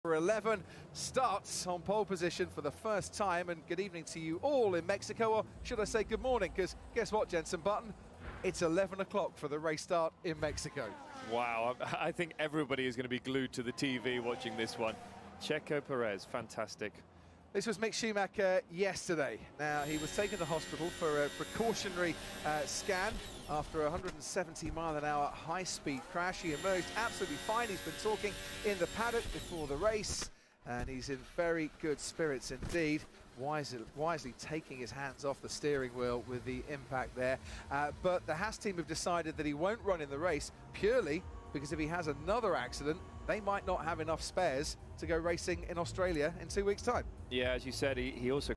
MICK SCHUMACHER'S QUALIFYING CRASH WITH NEW ANGLES
Onboard cam of Mick Schumacher's scary qualifying crash at Jeddah Street Circuit.